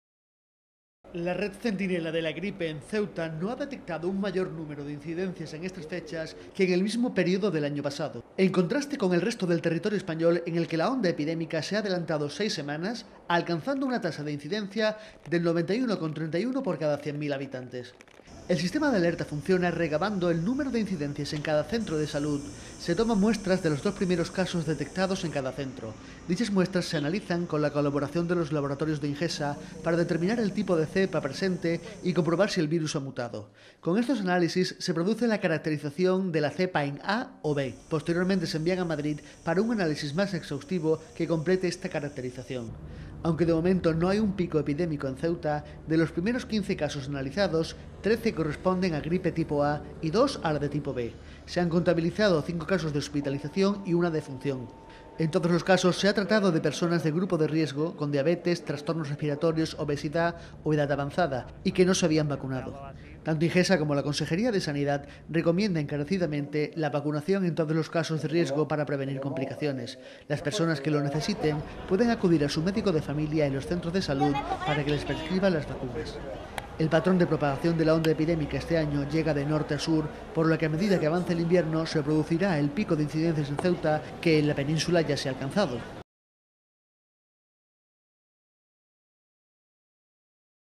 La epidemia de la gripe se adelanta en el resto de España pero no en Ceuta
Aunque las incidencias de la gripe indican que el presente año se ha adelantado el pico de la epidemia en el casi todo el territorio español, no ha ocurrido de igual manera en Ceuta que presenta un número de casos similar al del año anterior. Aún así se han registrado cinco hospitalizaciones debido a la gripe y un fallecimiento. En todos los casos se trataba de personas dentro de grupos de riesgo que no se habían vacunado.